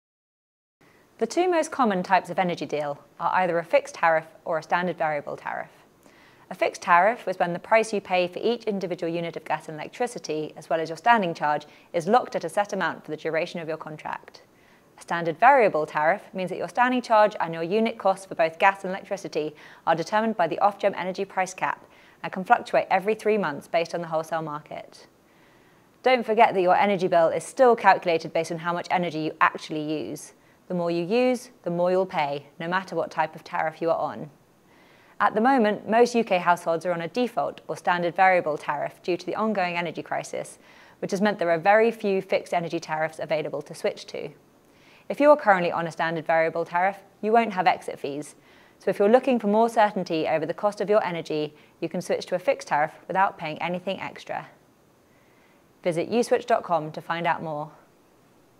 What is a fixed price energy plan?
Do you know what is a fixed energy plan, and how does that differ from a standard variable plan?